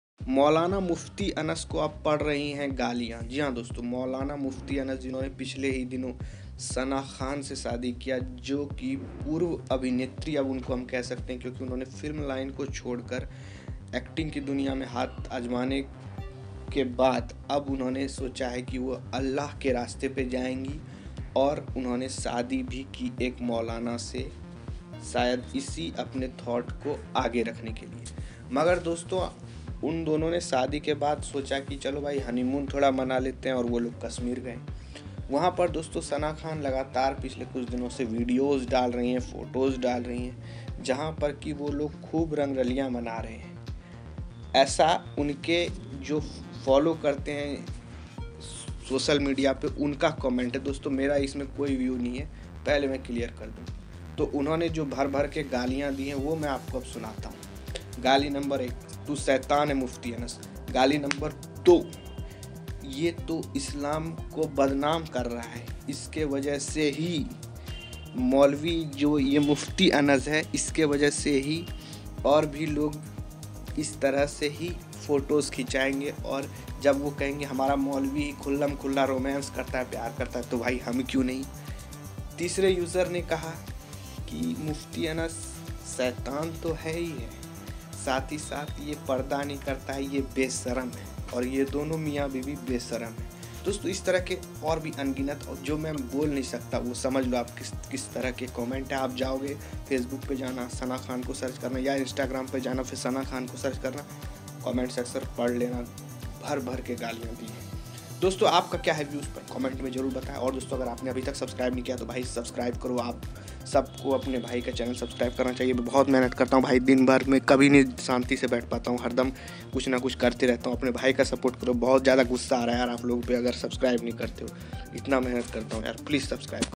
VIDEO: Sana Khan ke Shohar Mukti Anas ko Log De rhe Hai Bhar Bhar Ke Gali khud Dekhiye
video credit:- sana khan